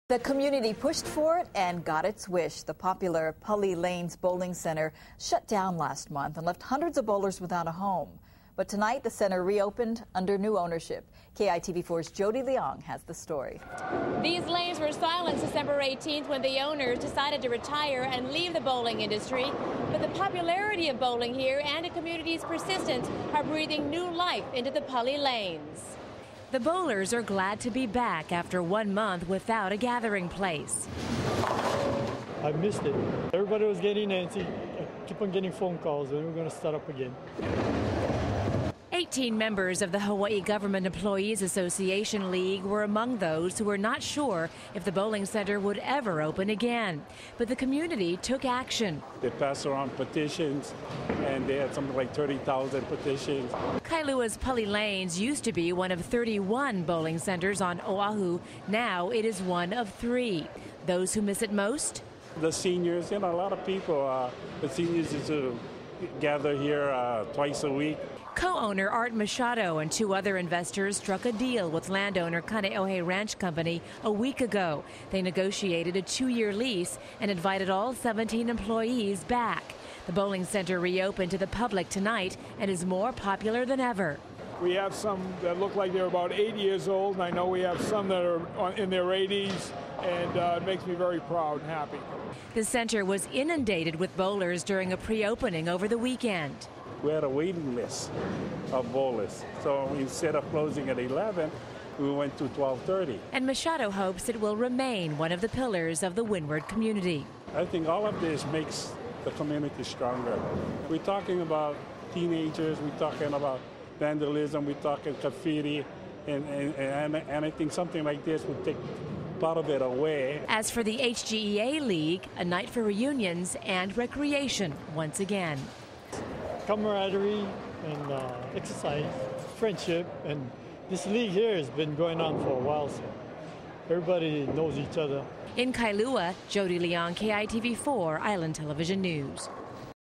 Pali Lanes Bowling Center Reopens
The popular Pali Lanes Bowling Center re-opened after a one month hiatus. 1/18/2010. For news you need to know,